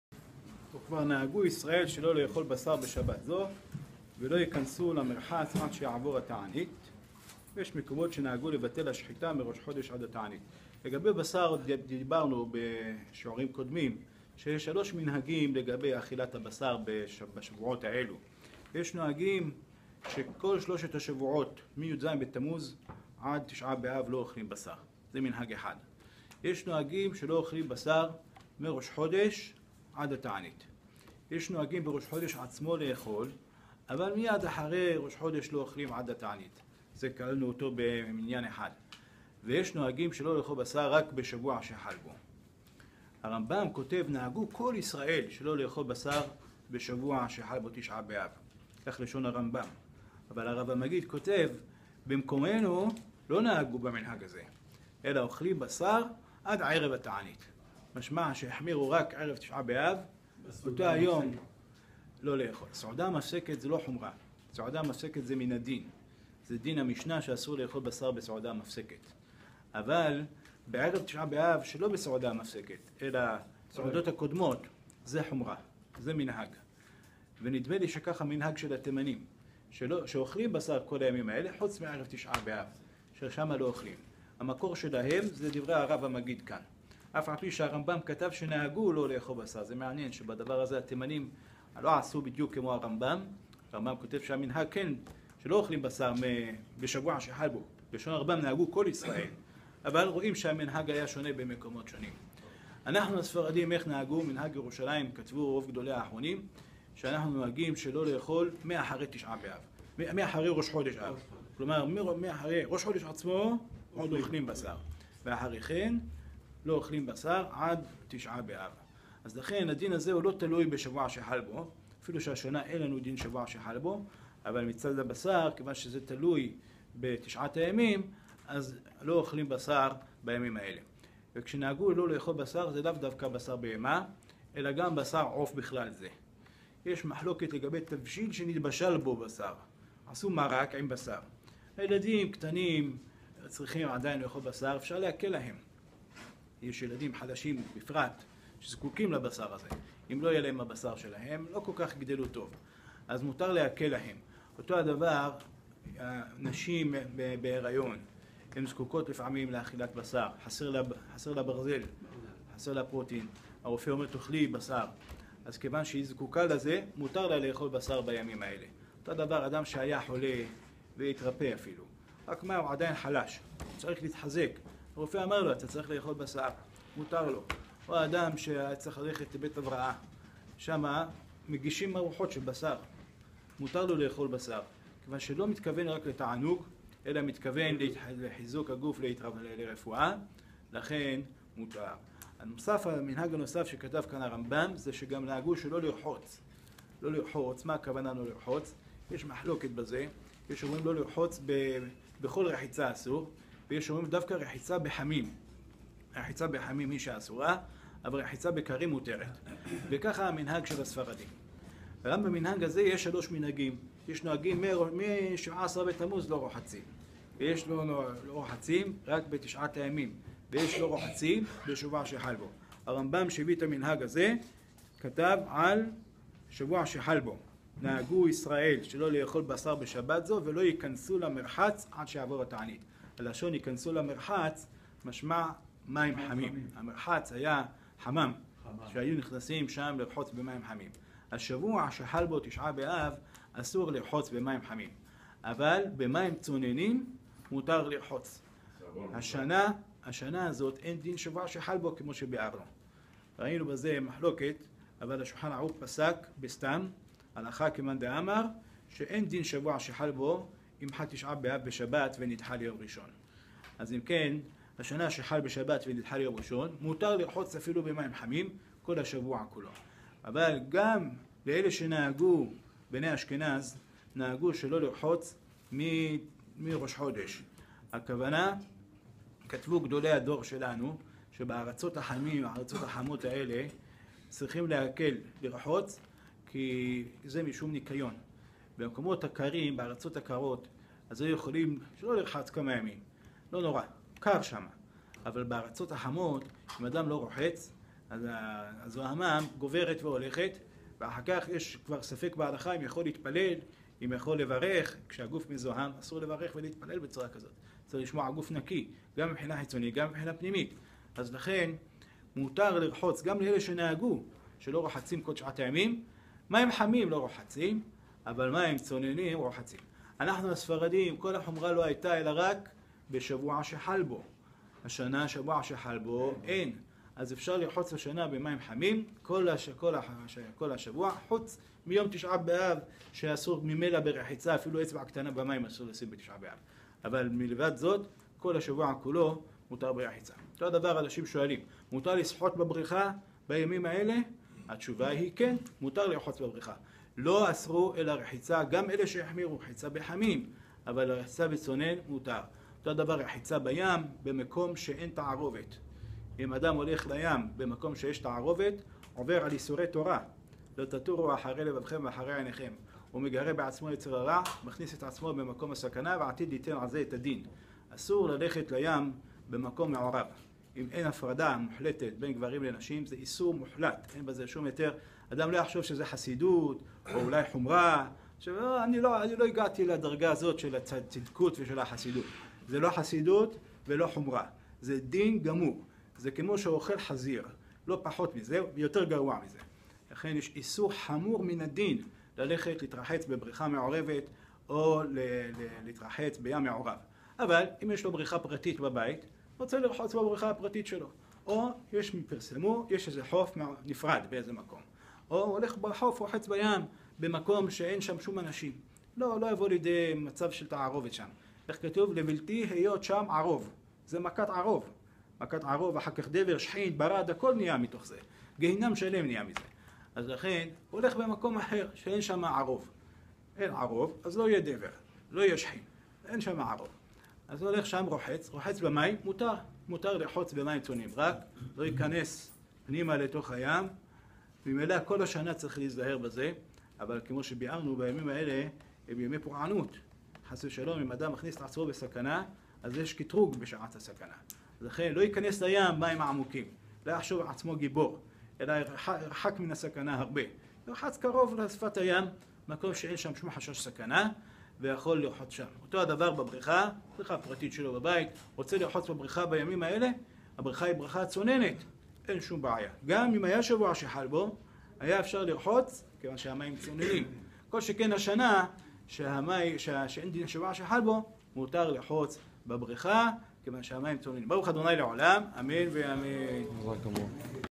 הלכות תשעת הימים - דיני אכילת בשר בתשעת הימים ורחיצה בשבוע שחל בו - הרב יצחק ישראלי
המנהגים השונים באכילת בשר עד תשעה באב. רחיצה, הליכה לבריכה ולים בימים אלו. איסור רחיצה בחוף או בריכה מעורבים והסכנה שבכך.
שיעור הלכה יומית מתוך רמב"ם הלכות תעניות פרק ה'
נמסר על ידי הרב יצחק ישראלי שליט"א
בבית הכנסת "שערי ישועה מציון", Mill Basin
כ' תמוז תשע"ה
שיעורי תורה נוספים להאזנה ולצפיה באתר: